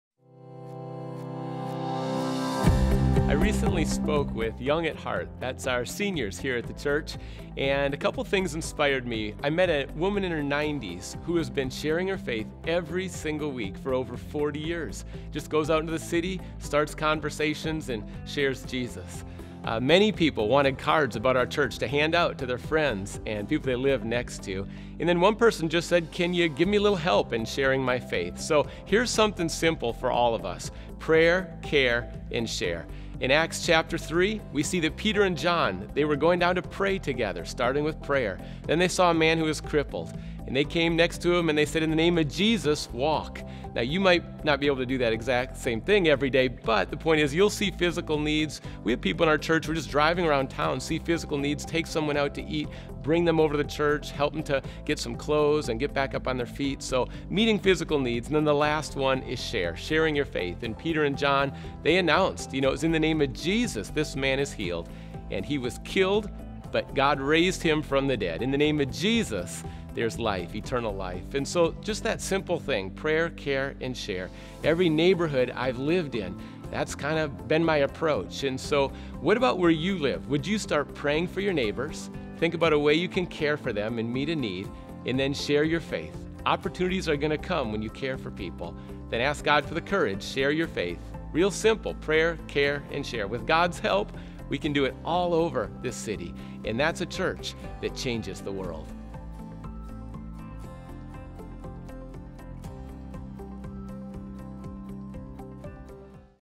Prayer, Care, and Share - Daily Grace 51
Read Acts 3:1-16 Are you reaching out to your neighbors? Prayer, care, and share is a simple and effective approach. Peter and John were devoted to prayer, met a physical need, and then led people to Jesus.